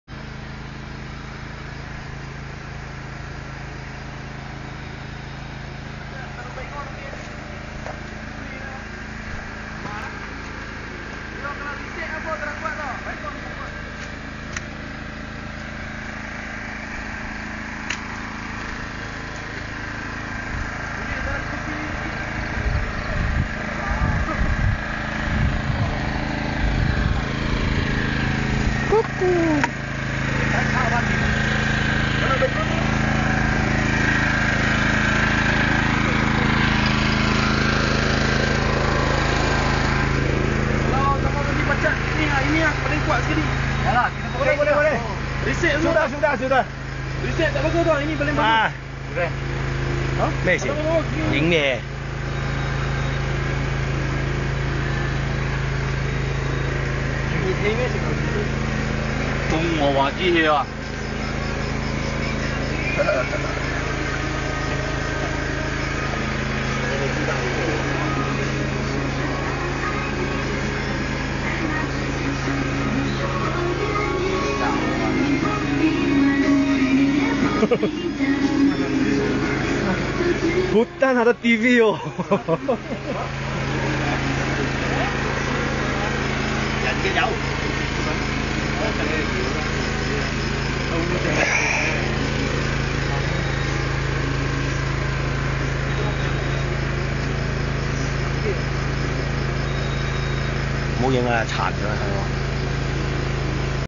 Off Road Janca baik 2010 01
night riding  Off road, champing .fishing..n visited some friends at jungle.